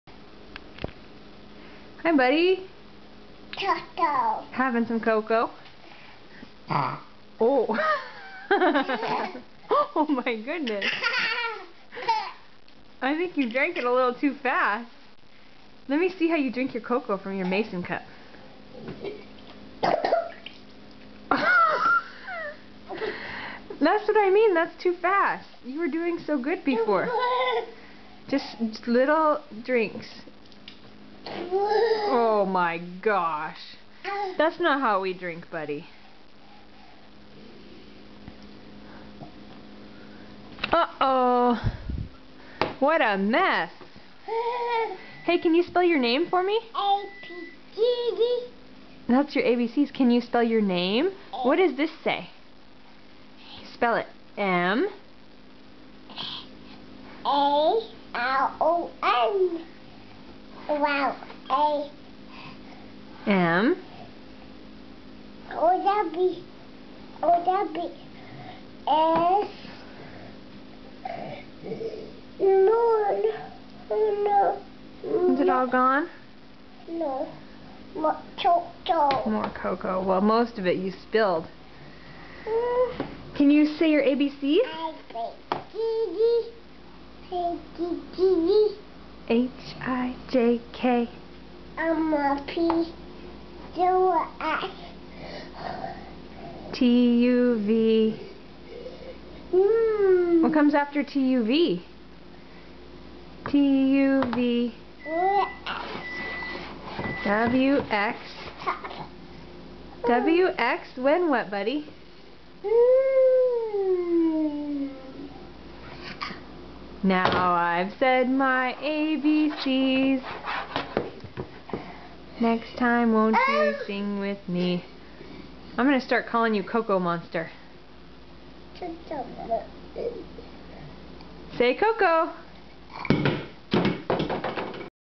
Cocoa Monster
Mason drinks cocoa and makes a mess...